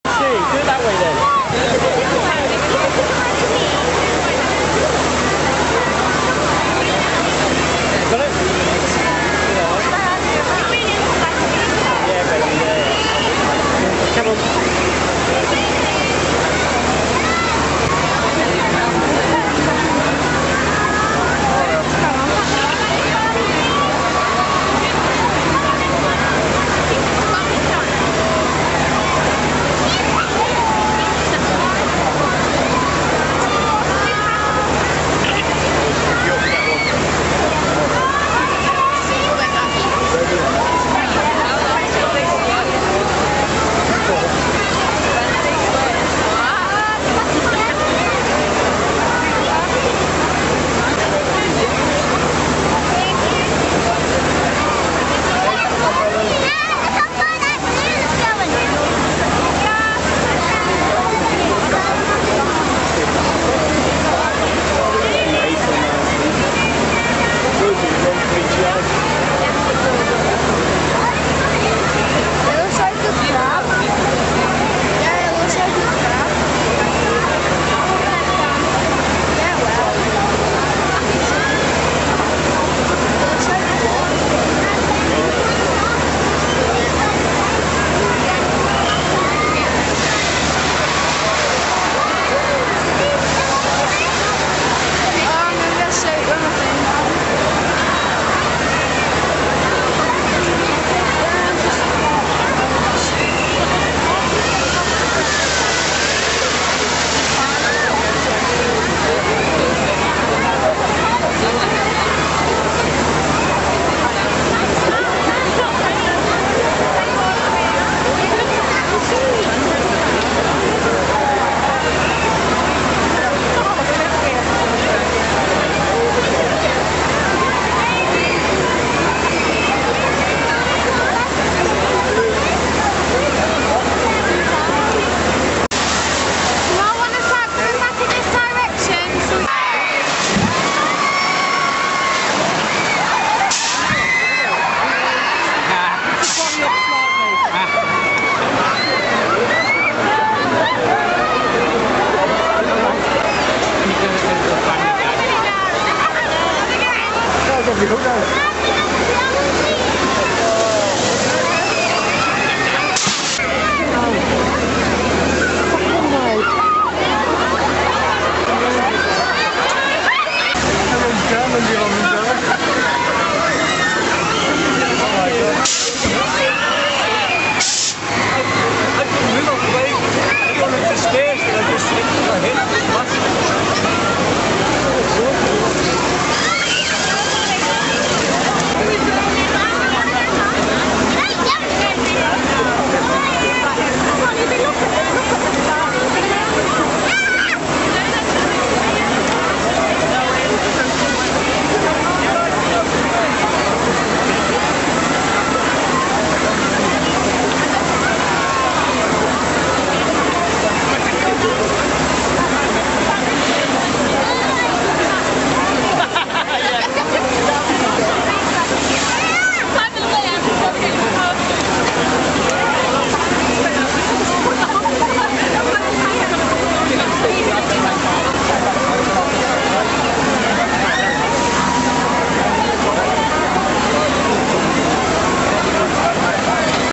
Derby Square Liverpool La Machine
La Princess Giant mechanical spider in Liverpool's Derby Square